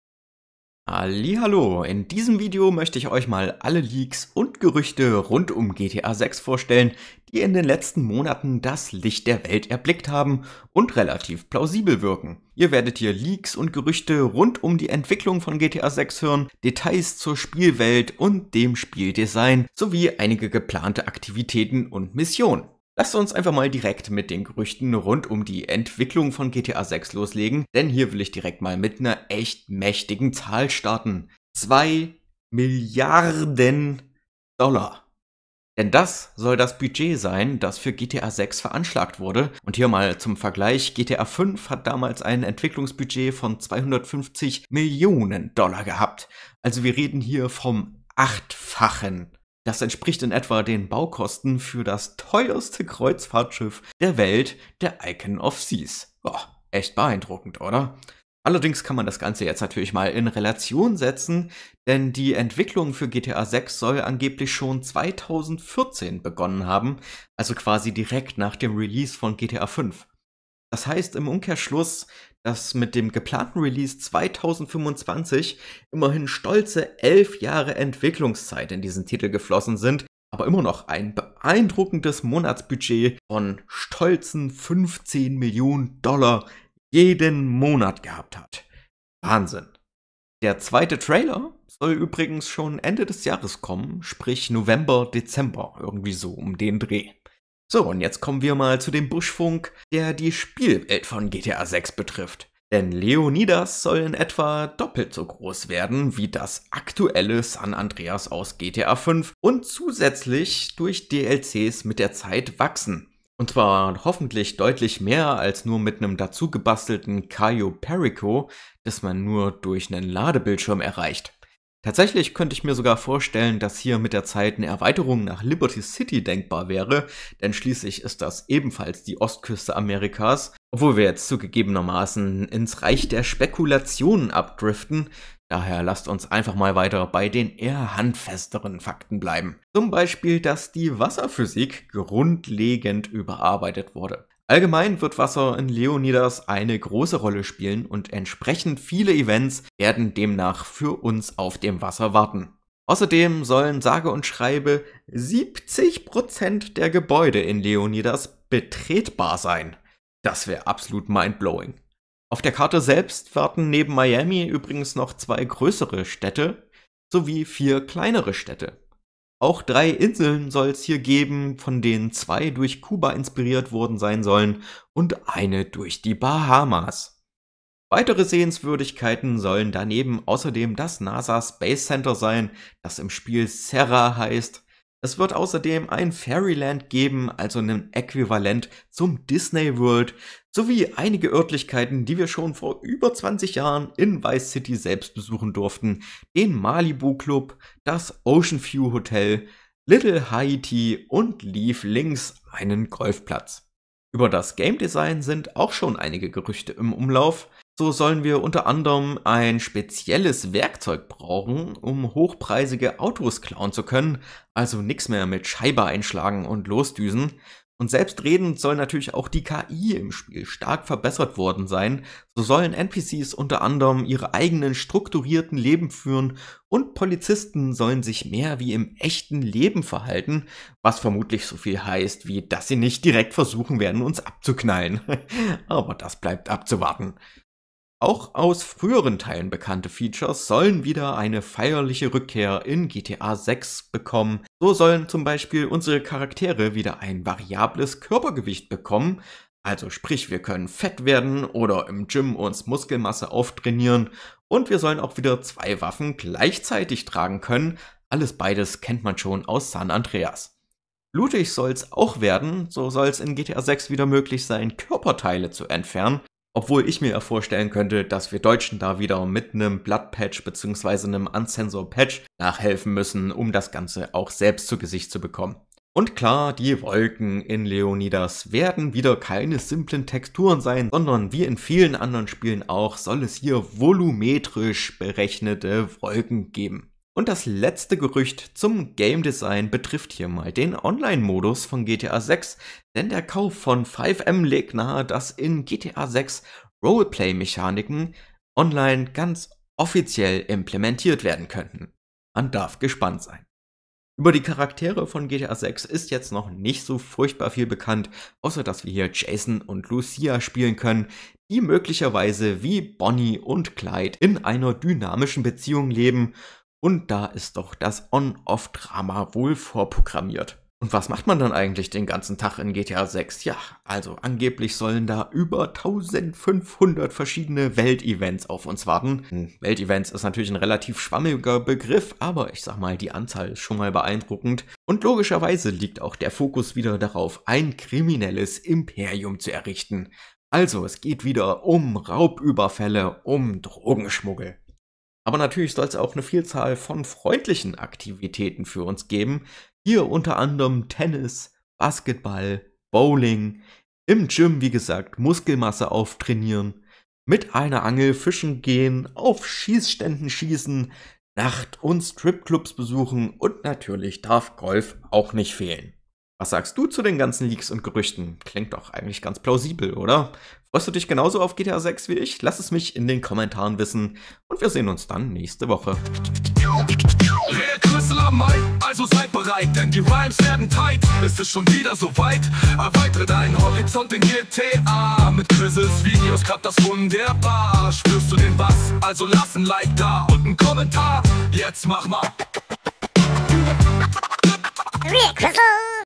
GTA VI: Alle aktuellen LEAKS und Gerüchte auf einen Blick!
In diesem Video erhältst du einen umfassenden Überblick über die neuesten Leaks und Gerüchte zu GTA VI! Erfahre alle spannenden Details zur Karte, den Spielmechaniken und der Entwicklung, die derzeit die Gerüchteküche zum Brodeln bringen.

🚨 GTA VI LEAKS & GERÜCHTE: Der aktuelle Stand (August 2024) 🚨

🔍 Was dich erwartet:

· Hintergrundinfos zur Entwicklung von GTA VI
· Spekulationen zur Spielwelt und den Mechaniken
· Alles, was wir bisher über die Hauptcharaktere Jason & Lucia wissen
· Mögliche Missionen und Aktivitäten im Spiel

Bitte beachtet, dass Rockstar Games bislang nur den offiziellen TRAILER zu GTA VI veröffentlicht hat und sich zu weiteren Details bedeckt hält. Alles, was ich euch in diesem Video präsentiere, basiert auf inoffiziellen Quellen, die ich für glaubwürdig halte und die meiner Meinung nach durchaus plausibel klingen. 🕵️‍♂️

💬 Eure Meinung zählt: Lasst mich wissen, was ihr von den Leaks haltet! Schreibt eure Gedanken und Spekulationen in die Kommentare – ich beantworte sie alle!

Ein riesiges Dankeschön an meine treue Community, die nicht nur zuschaut, sondern mal was in die Kommentare schreibt. Ihr seid die Besten! 🙌

Kapitelübersicht:
00:00 Intro
00:22 Hintergrundinfos
01:36 Details zur Spielwelt
03:24 Spielmechaniken
05:03 Jason & Lucia
05:21 Missionen & Aktivitäten